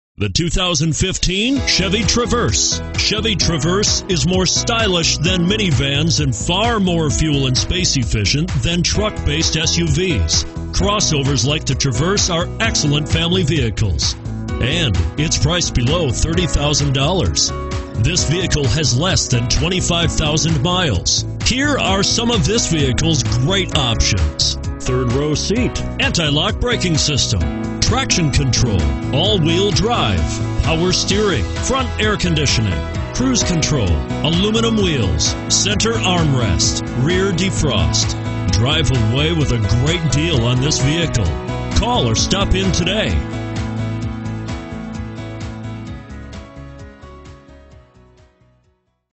2015 Chevrolet Traverse Minneapolis St. Cloud & Monticello MN 26019A
2015 Chevrolet Traverse LT

Looking for the right suv? Today could be your lucky day. With 24103 miles, this Black Granite Metallic, 2015 Chevrolet Traverse equipped with Automatic transmission could be yours. Request more information and set up a test drive right away.